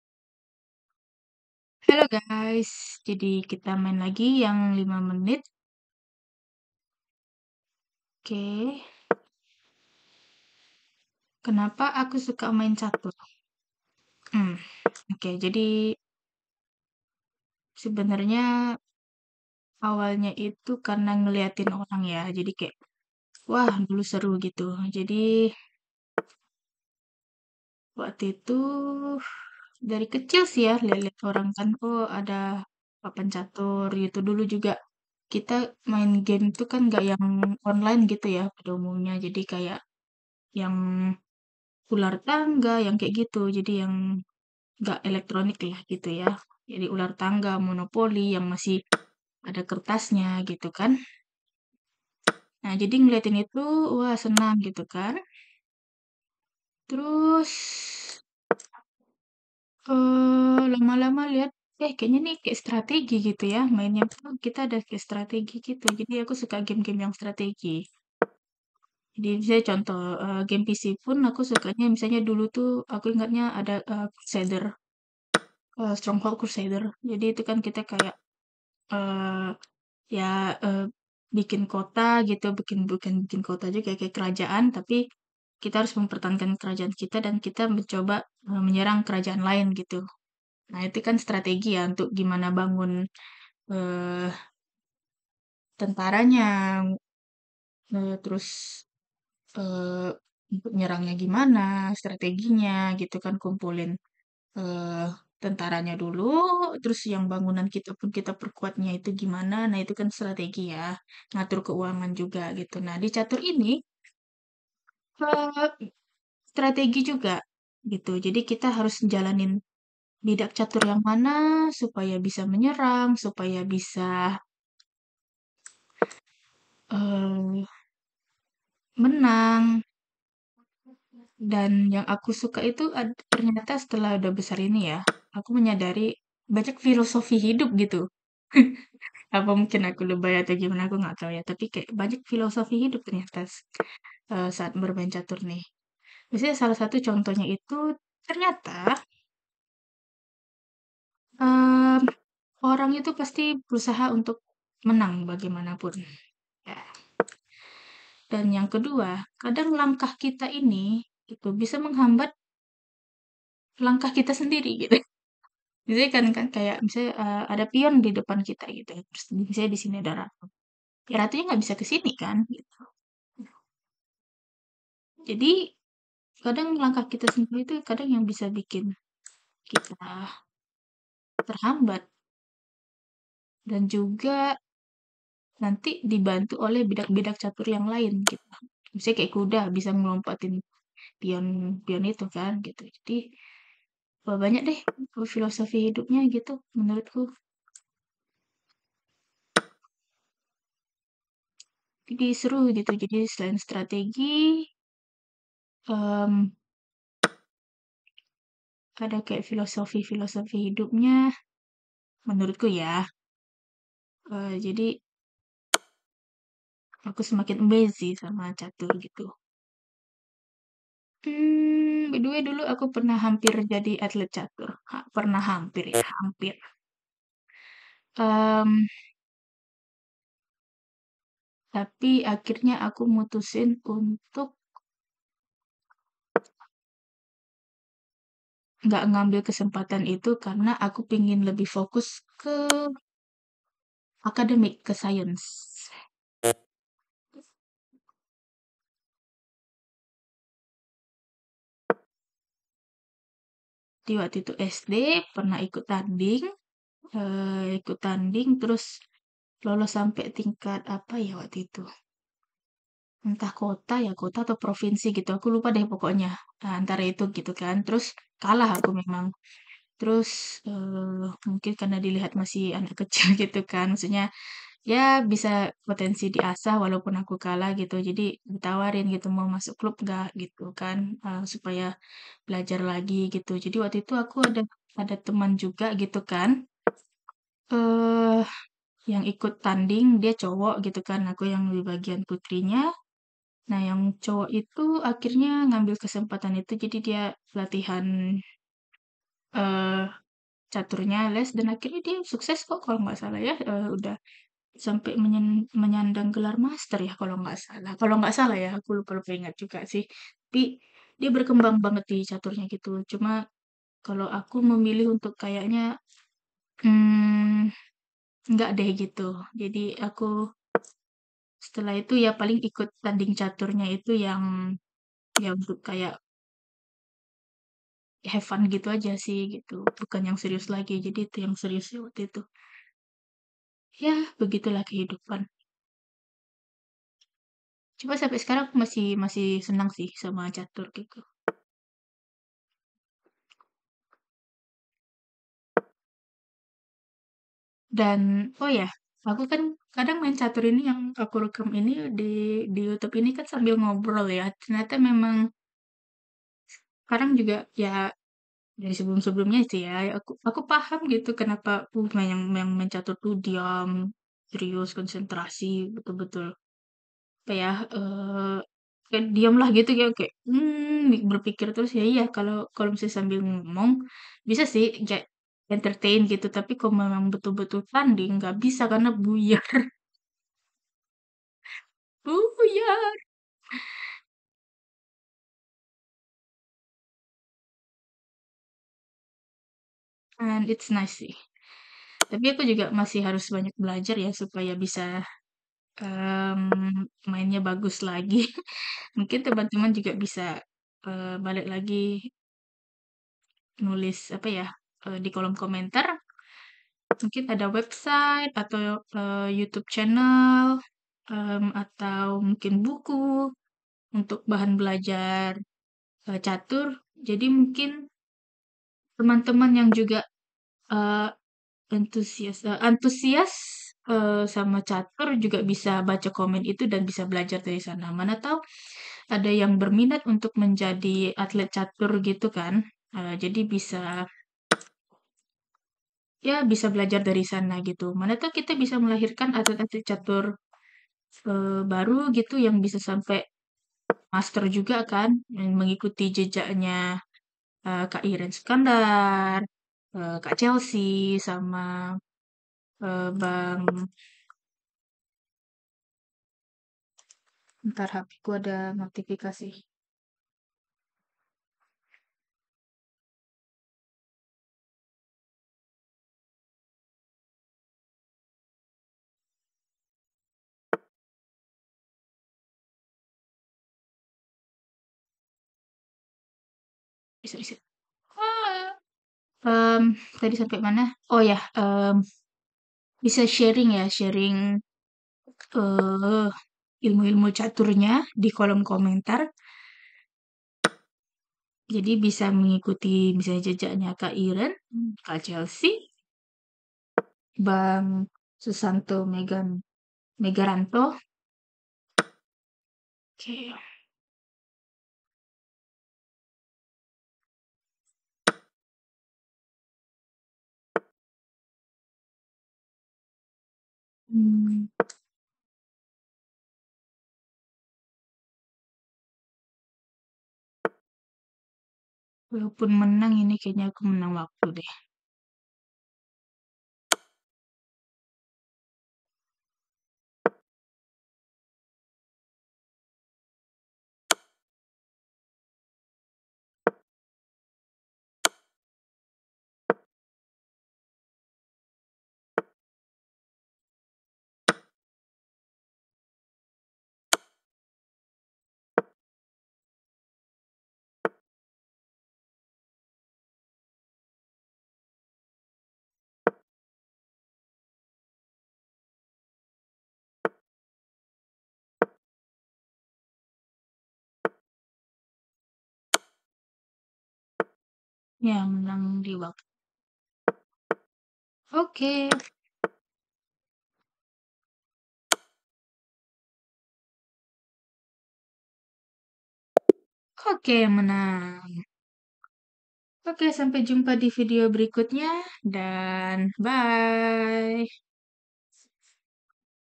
Main Catur yang 5 Menit Sambil Monolog
Main Catur sambil cerita-cerita. Ya... begitulah kisahnya.